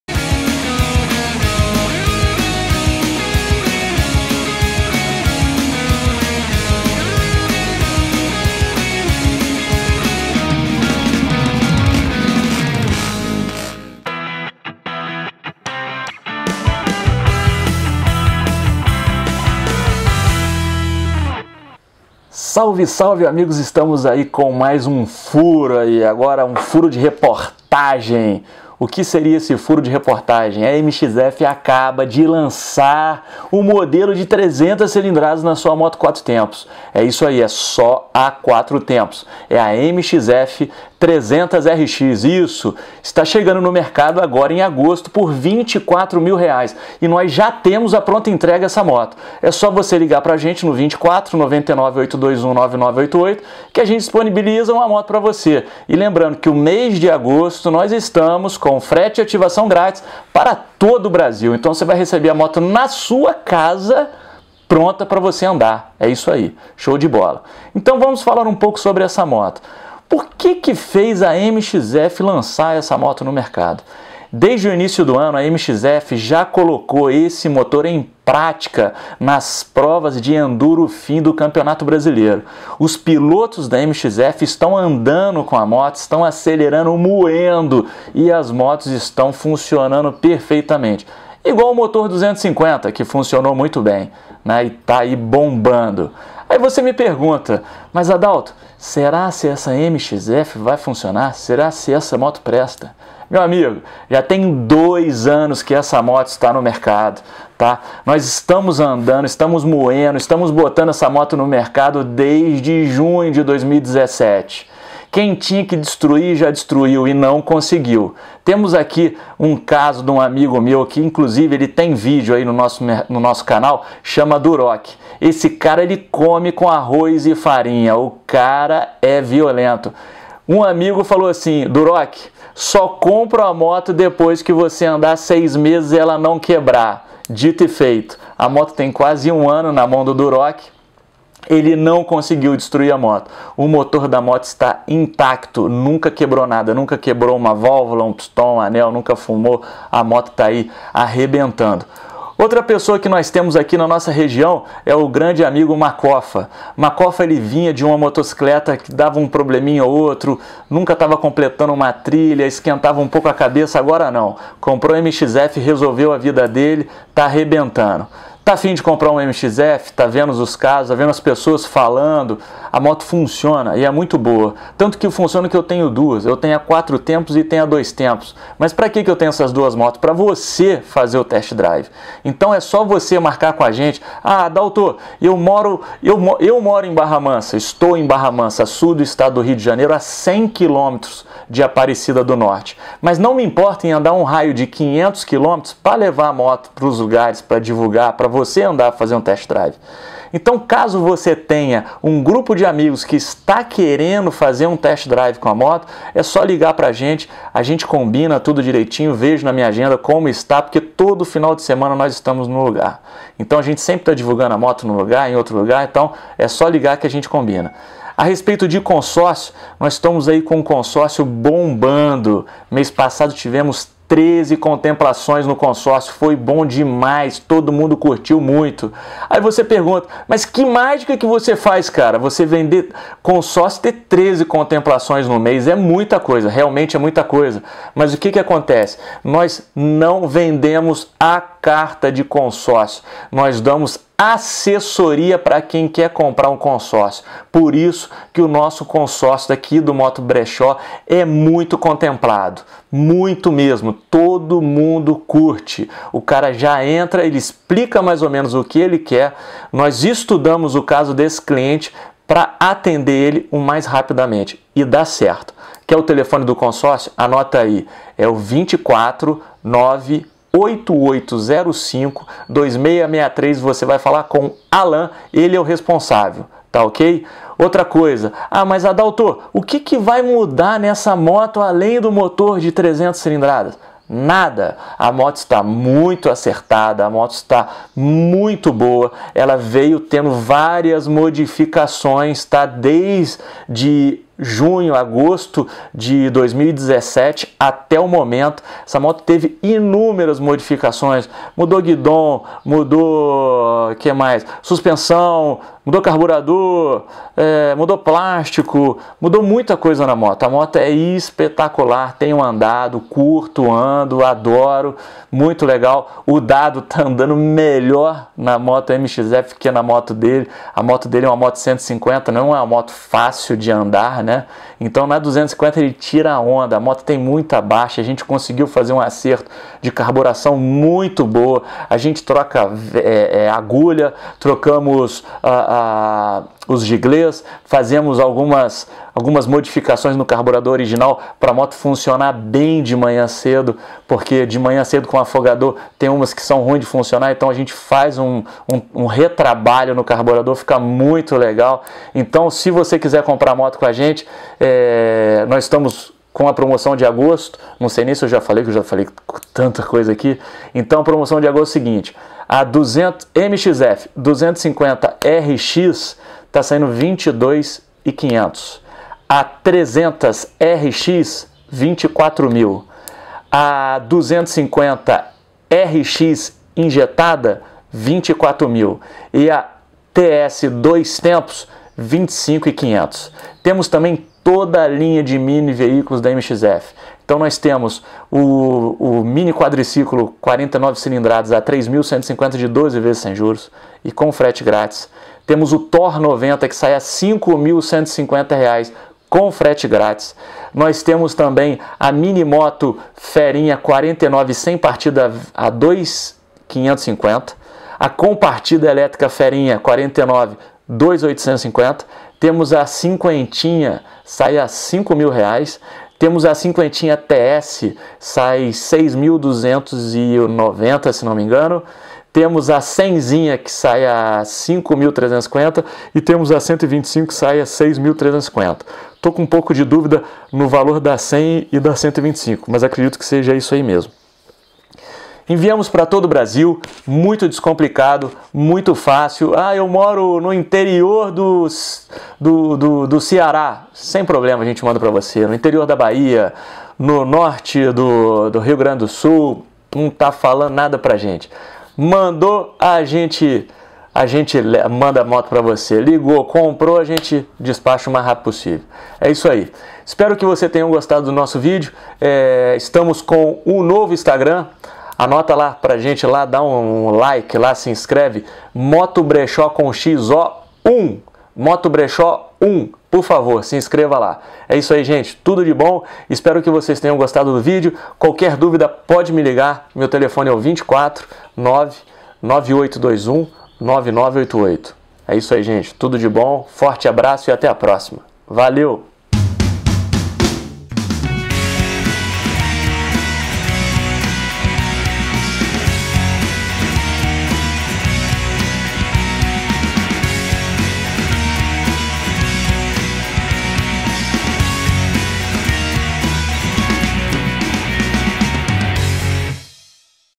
MXF 300 RX! Lançamento! VEJA O VIDEO!
A MXF acaba de lançar a sua moto de 300cc, sim a 250 oumentou de tamanho, agora esta com mais apititie, e de 28cv passa para 32, e o que ja era bom ficou ainda melhor!

Clique para saber mais sobre o consorcio, ja cai direto no ZAP